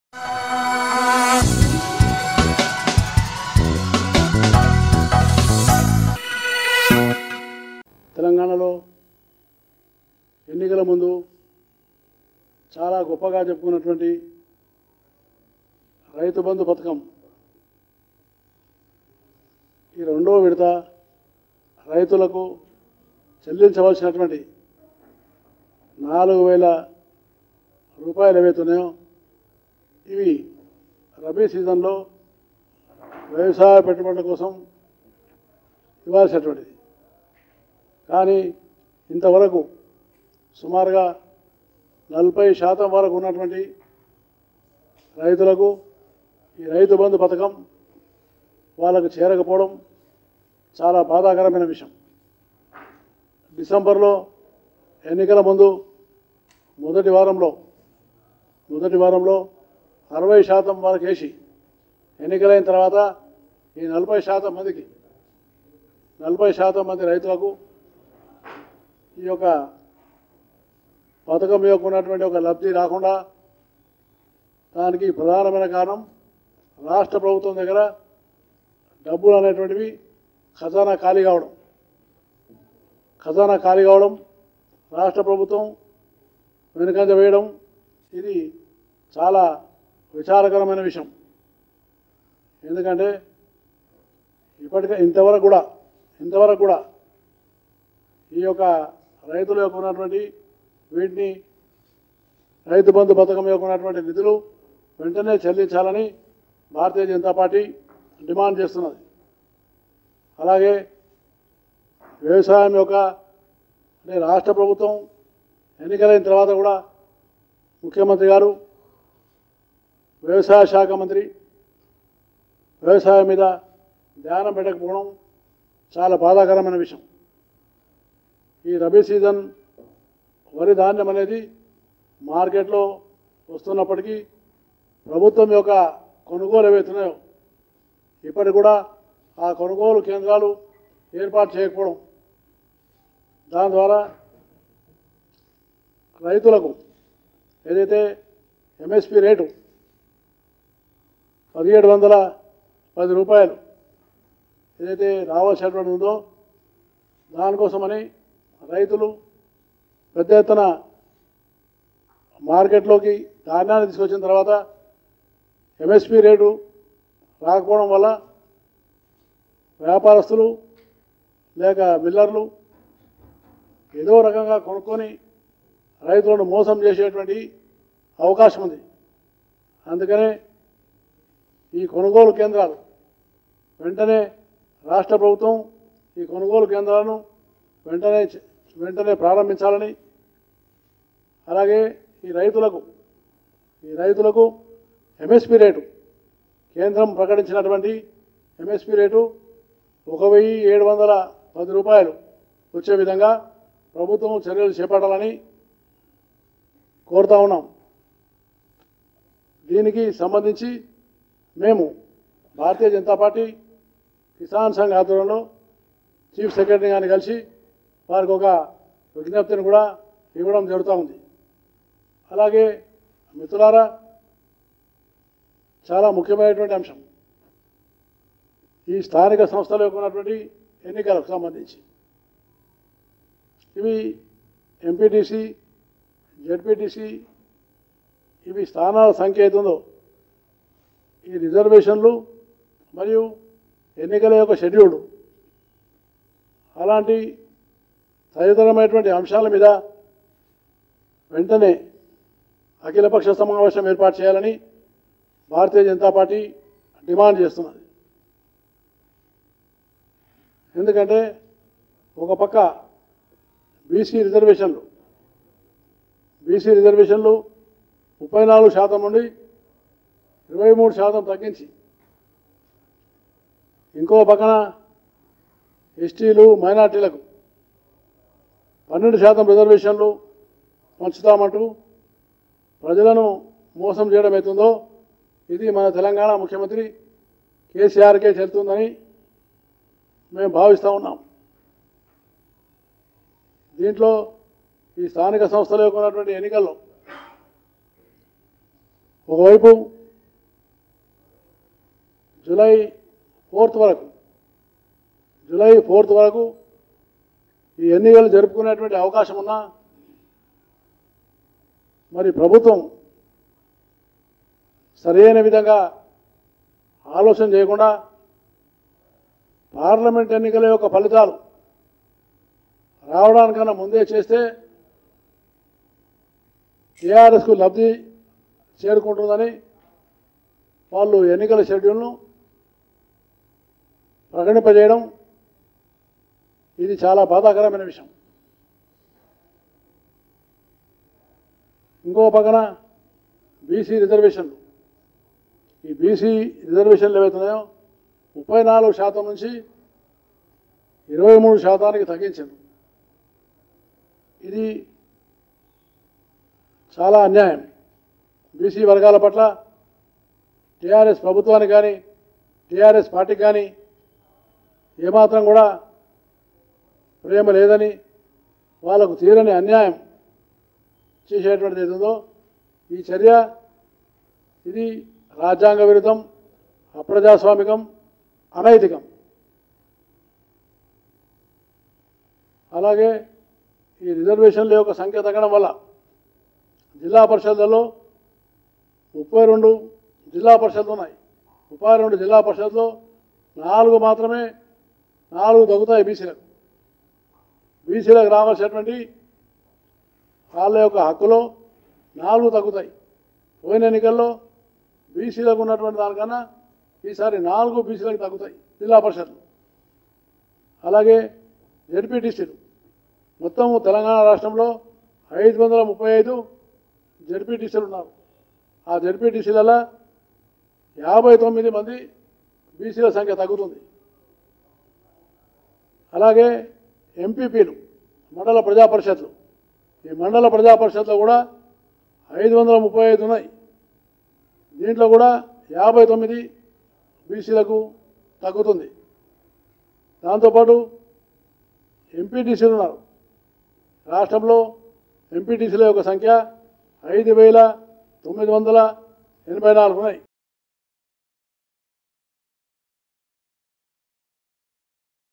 BJP Senior Leader Bandaru Dattatreya Press Briefing at BJP State Office, Hyderabad | Overseas News
BJP Senior Leader Bandaru Dattatreya Press Briefing at BJP State Office, Hyderabad